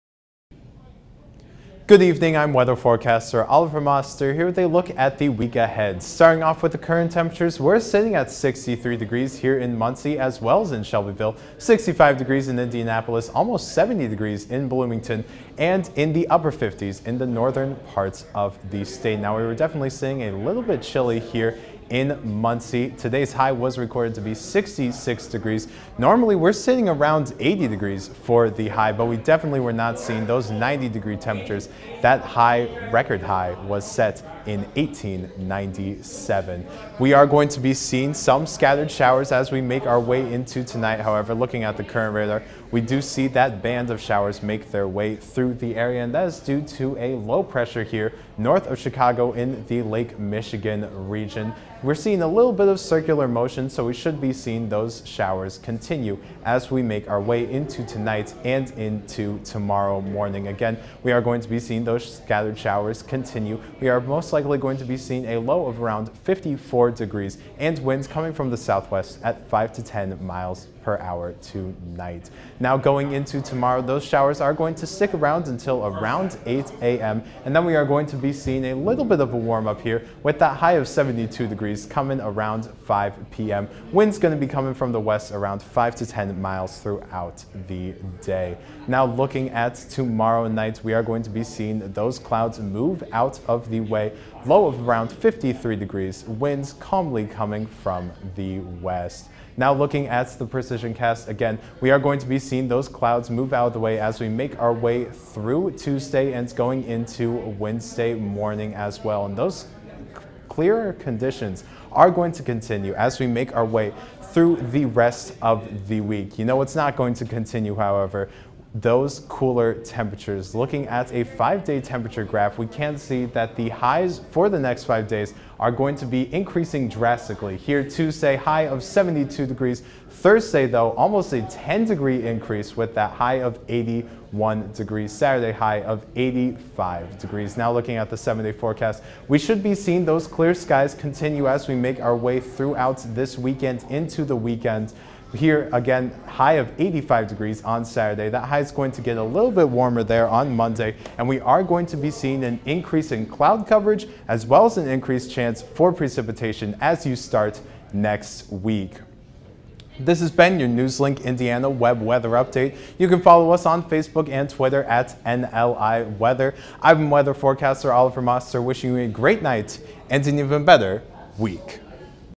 NewsLink Indiana Weather September 12, 2022 - Oliver Moster
Weather Forecaster Oliver Moster expects a warm up this coming week.